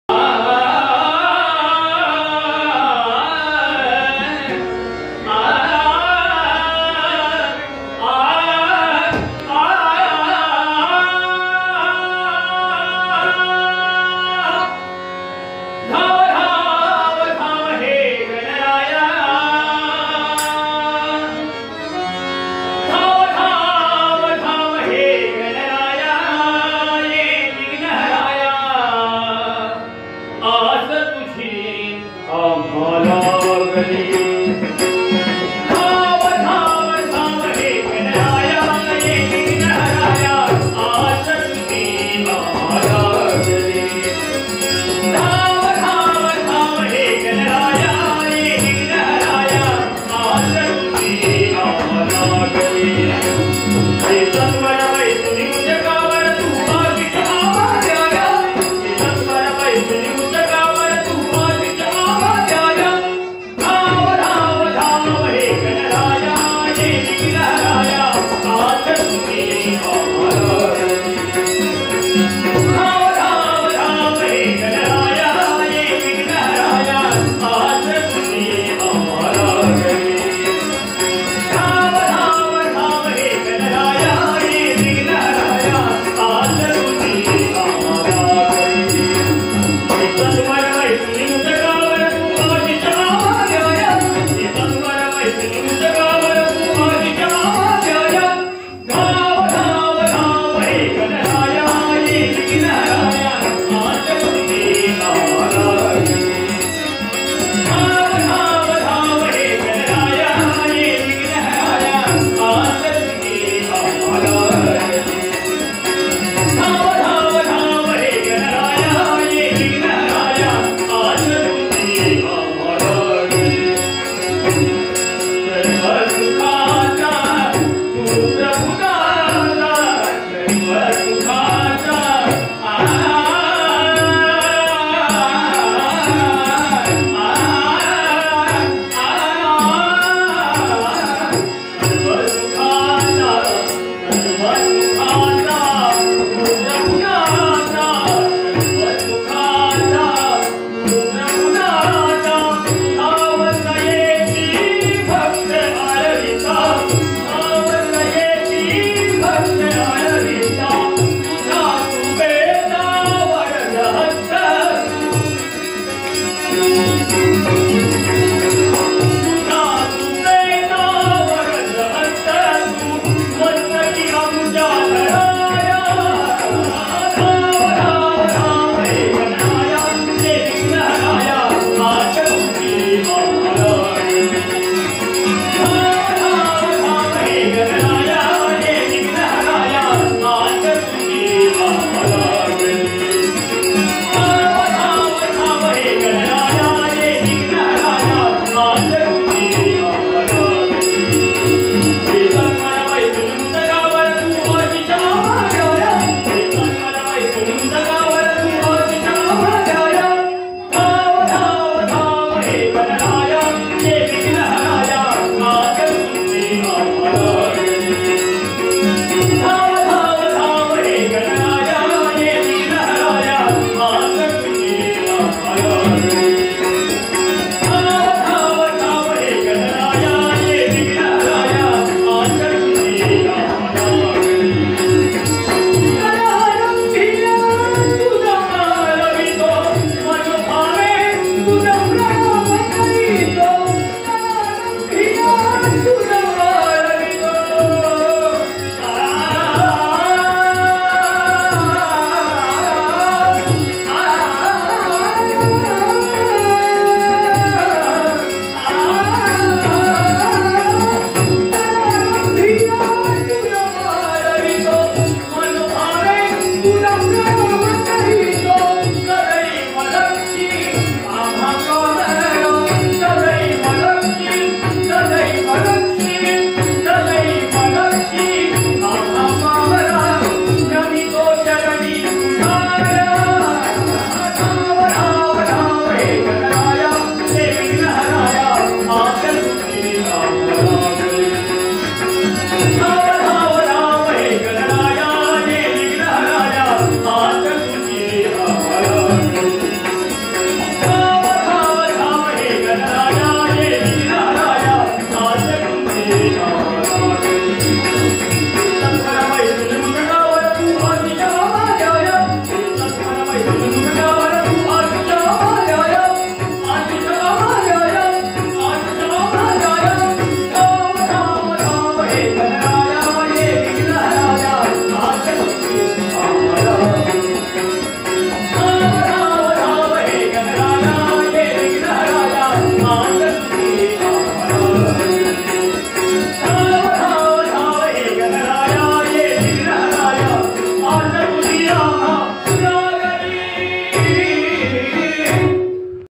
लॉकडाऊन नंतर पहिल्यांदाच सुप्रसिद्ध श्री खानोलकर दशावतार नाटक - श्री भैरव मंदिर कुडाळ 2021
हार्मोनियम श्री पप्पू घाडीगावकर
पखवाज श्री मेस्त्री